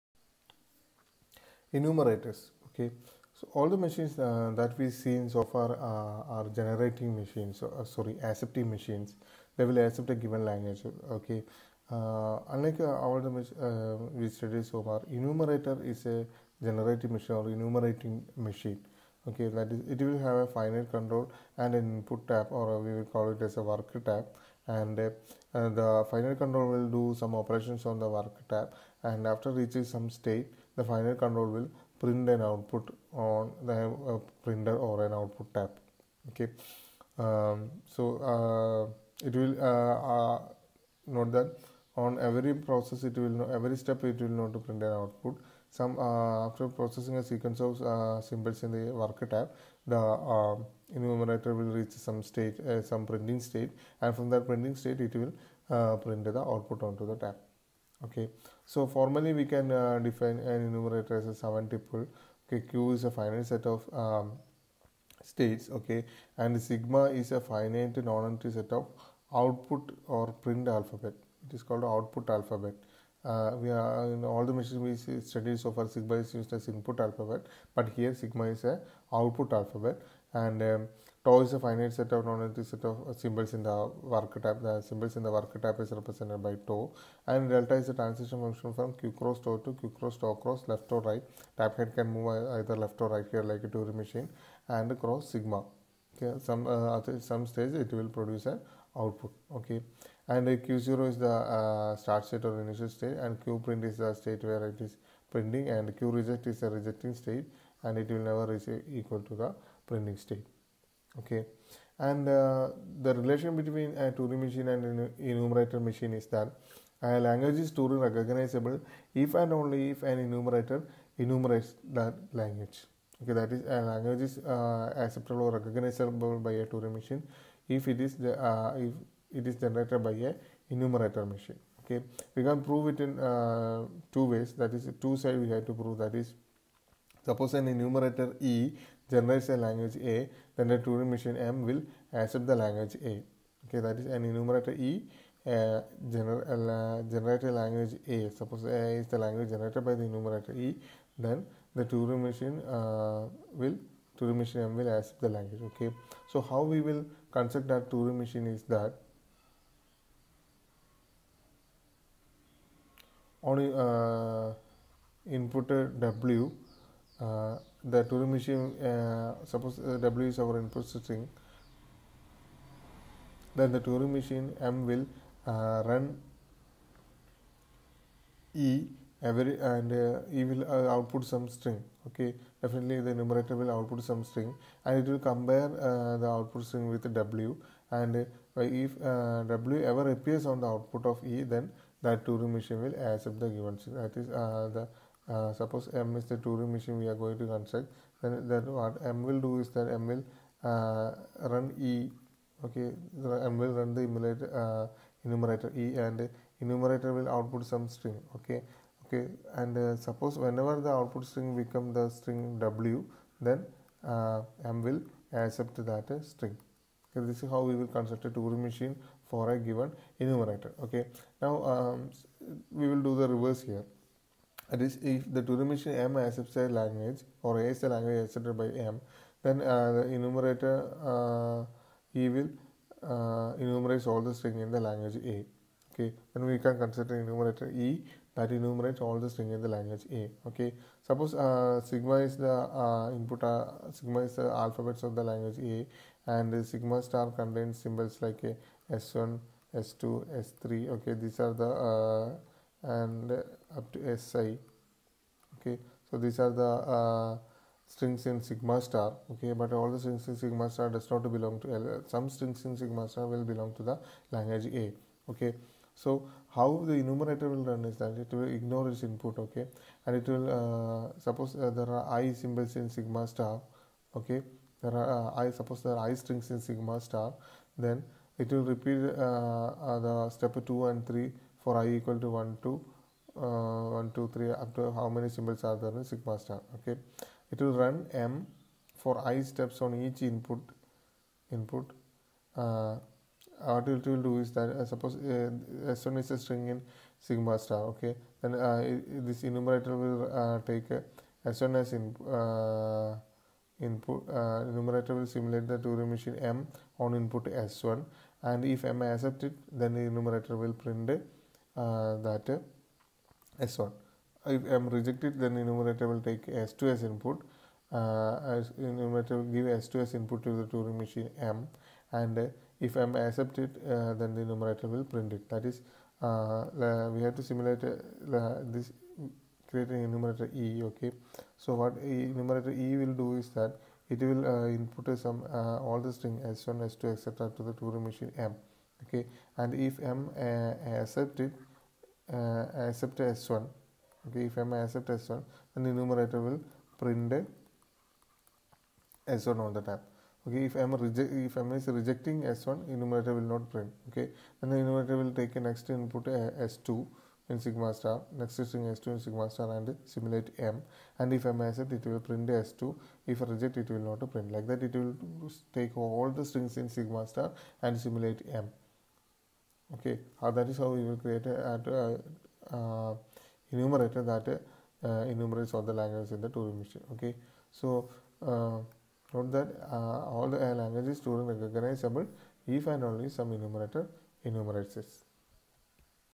Enumerator machine
Turing machine can act as an enumerator machine.